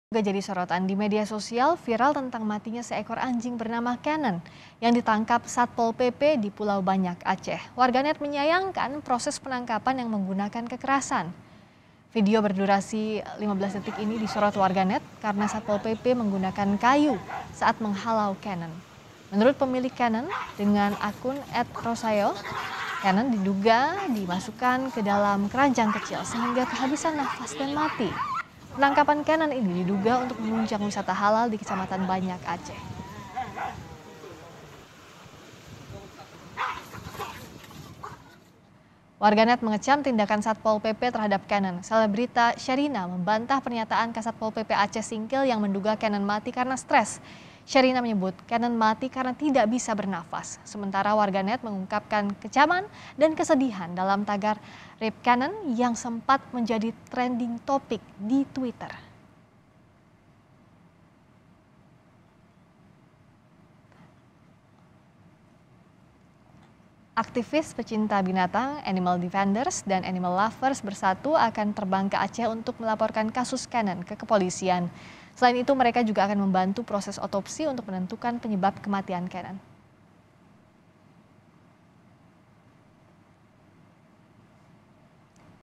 Canon Mati Usai Ditangkap Satpol PP Aceh
Netizen menyayangkan proses penangkapan yang menggunakan kekerasan.

Video berdurasi 15 detik ini disorot netizen karena Satpol PP menggunakan kayu saat menghalau Canon.

Menurut pemilik, Canon dengan akun @rosayeoh, Canon diduga dimasukkan ke dalam keranjang kecil sehingga kehabisan nafas dan mati.

Penangkapan Canon ini diduga untuk menunjang wisata halal di Kecamatan Banyak, Aceh.

Netizen mengecam tindakan Satpol PP terhadap Canon.

Selebrita Sherina membantah pernyataan Kasatpol PP Aceh Singkil yang menduga Canon mati karena stress.

Sherina menyebut, Canon mati karena tidak bisa bernafas.

Sementara, netizen mengungkapkan kecaman dan kesedihan dalam tagar RIP Canon yang sempat menjadi trending topik di Twitter.

Aktivis pecinta binatang Animal Defenders dan Animal Lovers Bersatu akan terbang ke Aceh untuk melaporkan kasus Canon ke kepolisian.

Selain itu mereka juga akan membantu proses otopsi untuk menentukan penyebab kematian Canon. (*)